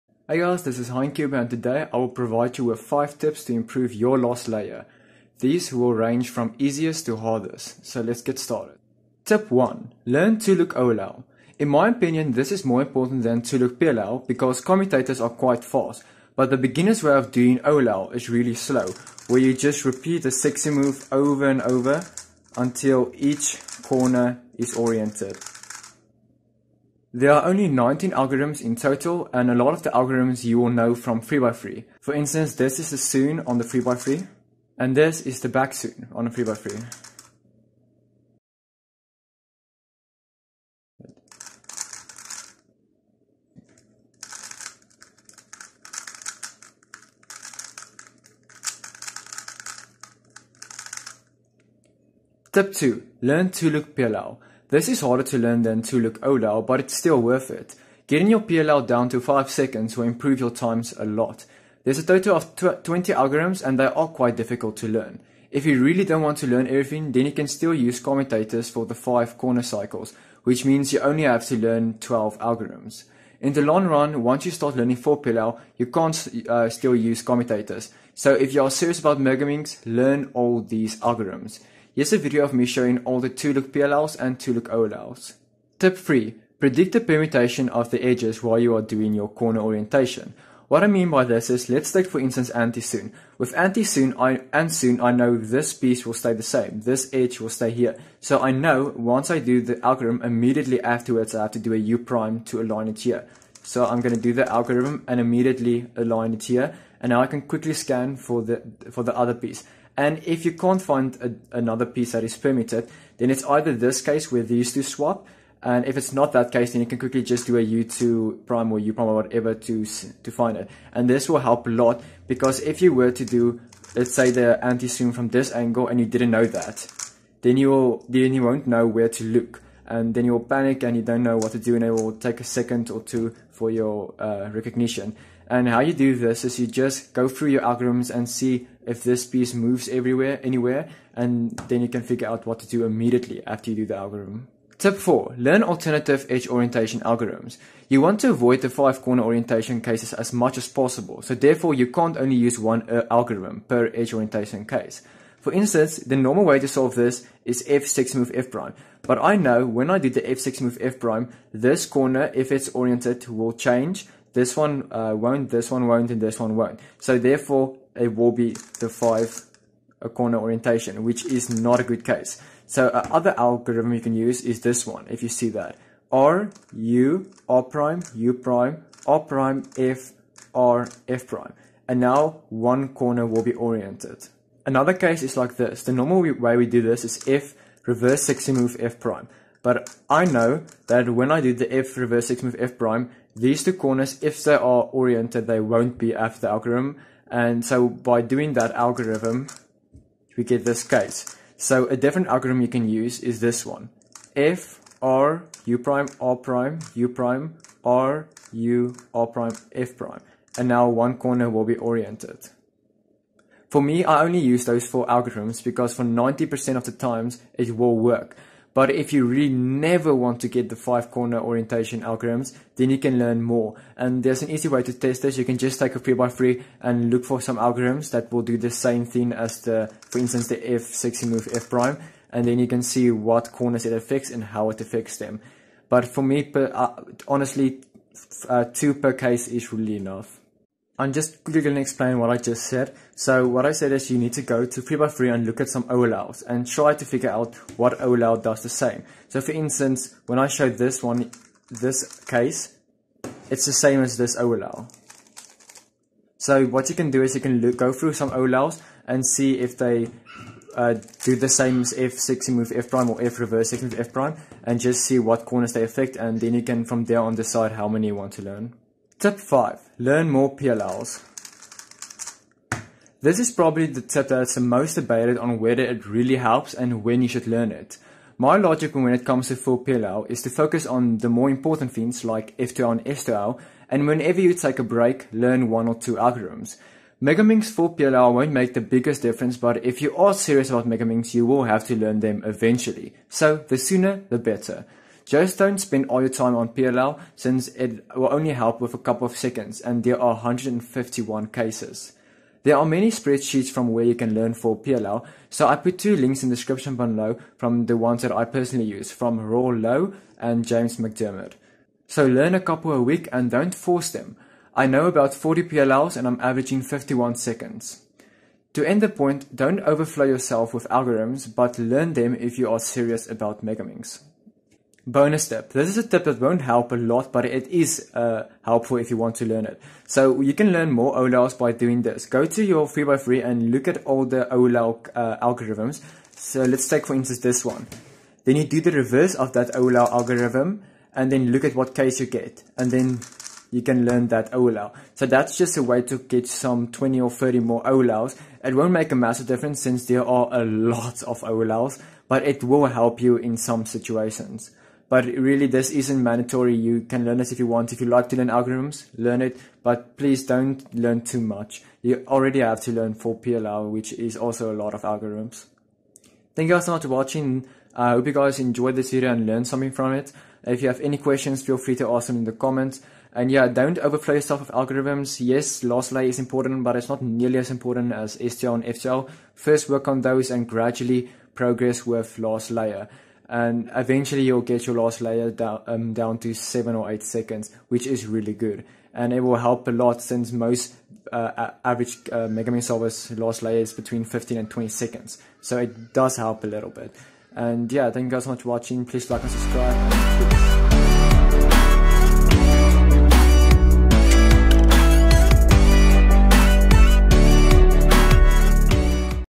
5 Megaminx Last Layer Tips (Easy to Advanced)
Hey this video was made to help you imrpove your last layer on Megaminx. The tips range from beginners to advanced but you will still learn something new even if you are a beginner.

Time stamps :
Tip(1) : 0:08
Tip(2) : 0:52
Tip(3) : 1:26
Tip(4) : 2:24
Tip(5) : 4:28